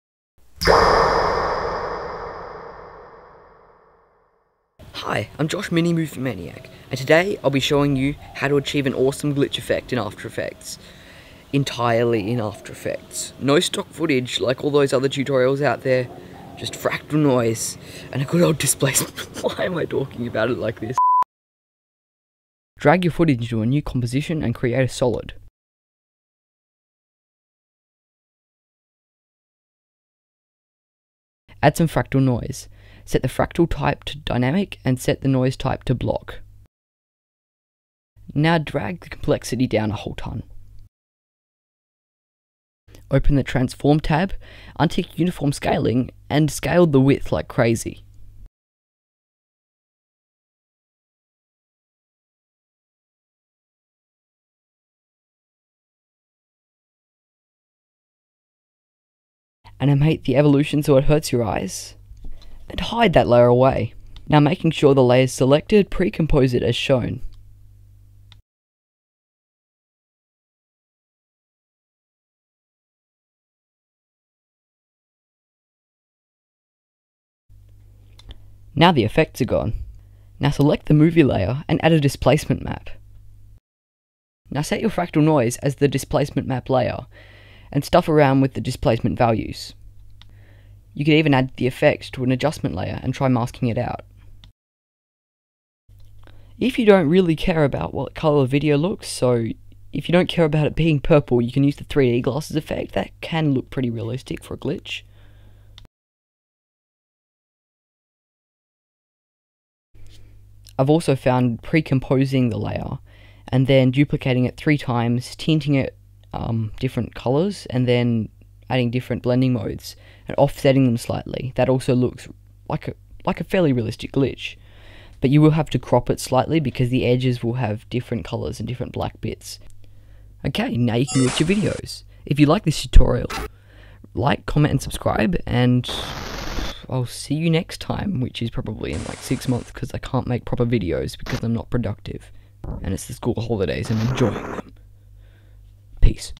How to Glitch Your Videos!! | After Effects Tutorial
Quick and easy glitch effect, and no stock footage needed. By a person with a short attention span for people with short attention spans :)

When I am talking about the 3D glasses effect you will see a little annotation pop up pointing to one of the options. This type of 3d effect was used in the thumbnail and retains the original colours, and looks great as a glitch. Consider it before you do the last method of colour separation as it takes a lot less time.